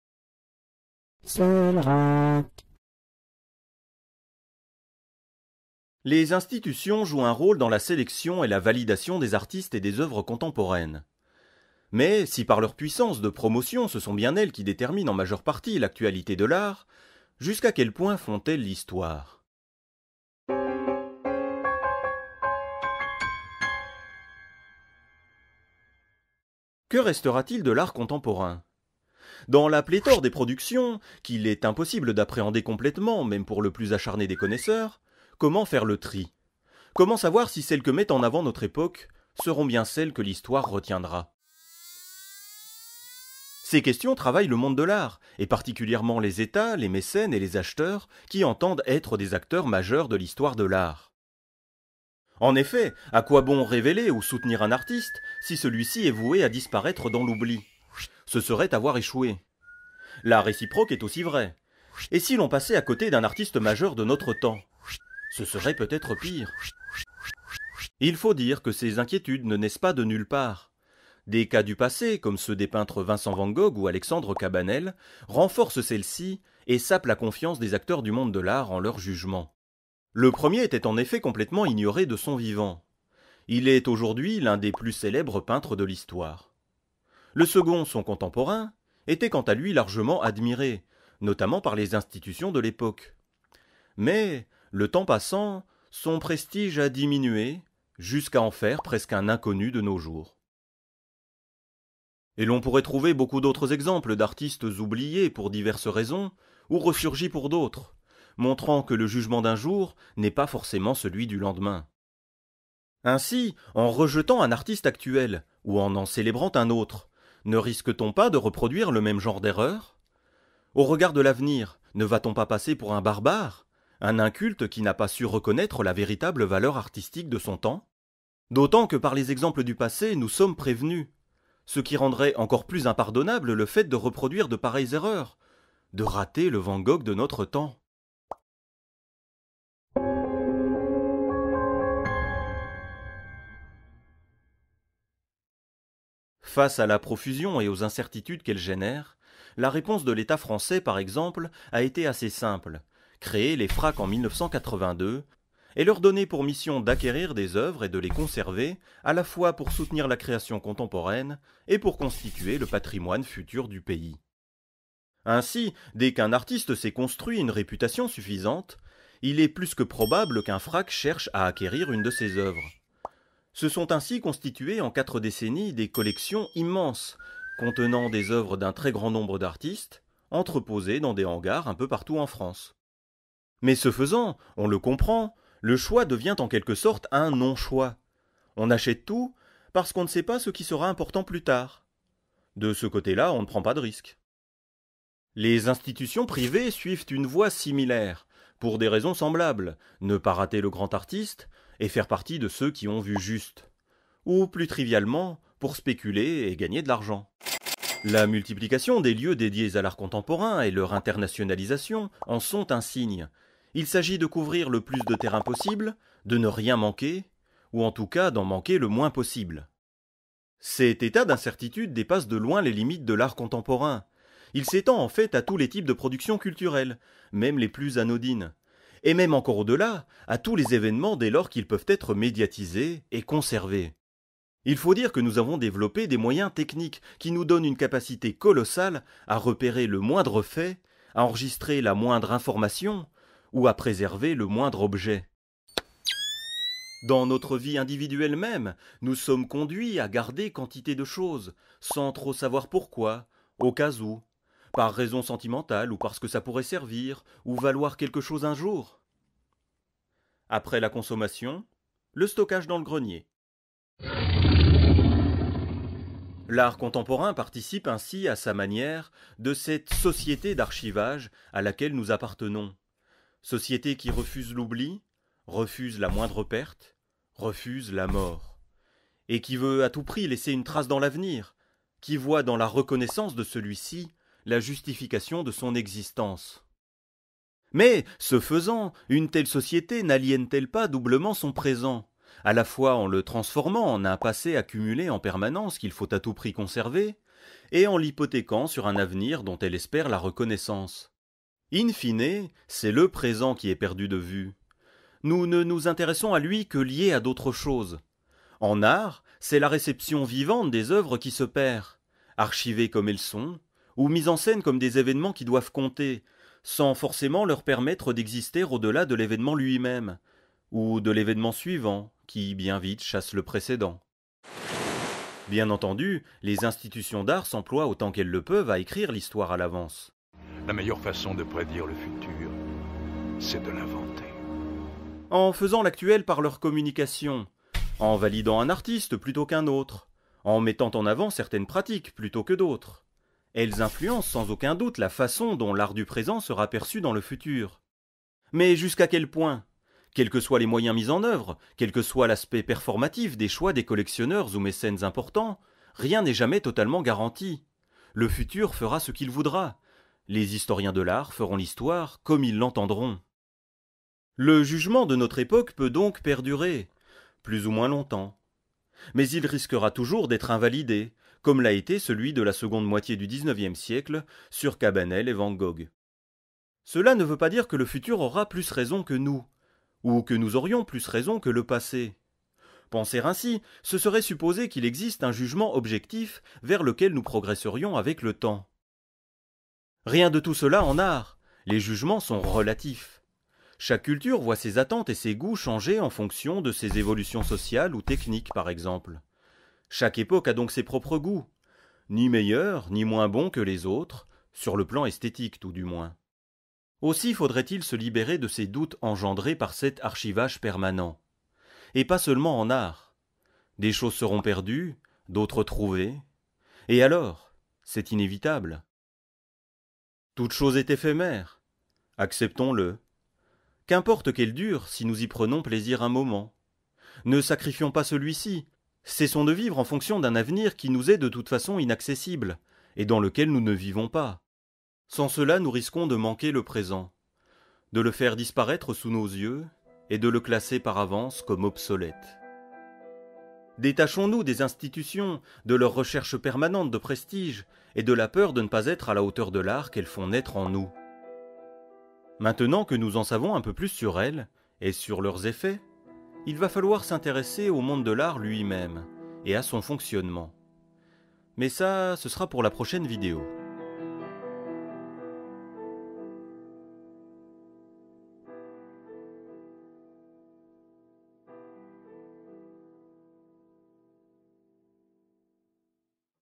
40- "Art contemporain". Onzième partie : le poids de l'avenir.
L’art célébré aujourd’hui sera-t-il celui qui restera dans l’histoire ? Tentons d’apporter des éléments de réponse dans cette vidéo qui forme, avec les précédentes, une sorte de chapitre sur les institutions à l’intérieur du cycle sur l’art contemporain.

Lorsqu'elles ne sont pas produites par moi-même, les images proviennent de wikimedia commons.